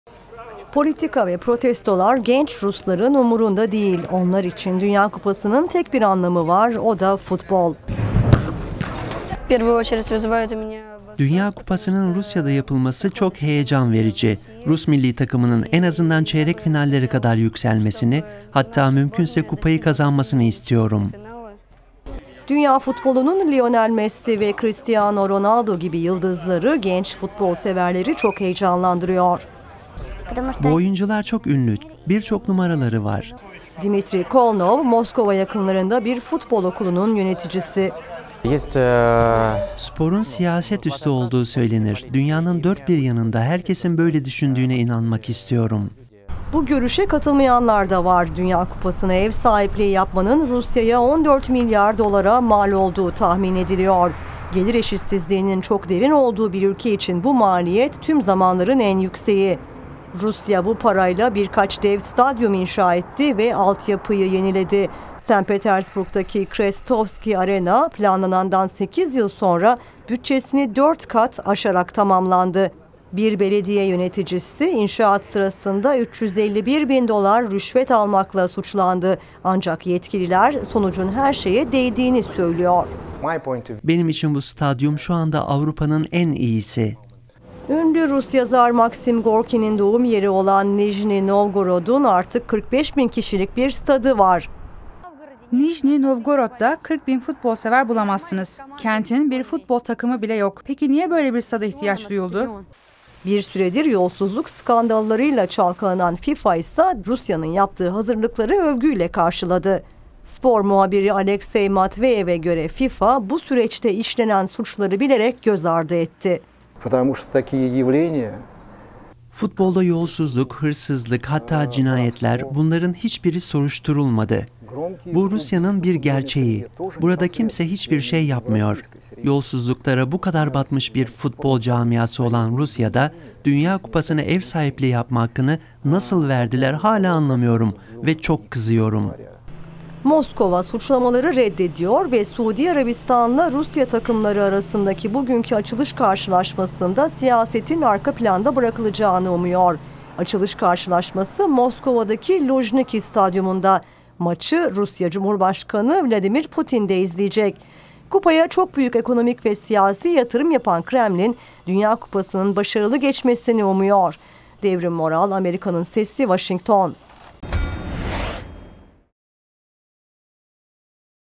Rusya Dünya Kupası’na Ne Kadar Hazır?
Rusya, Dünya Kupası’na ev sahipliği yapma hakkı kazandıktan sekiz yıl sonra bir ay sürecek karşılaşmaları başlattı. Ancak Dünya Kupası bütçesini 12 kat arttırması ve stadyum inşaatlarında yolsuzluklar yapıldığı iddiaları, “Rusya Dünya Kupası’na ne kadar hazır?” sorusunu gündeme getiriyor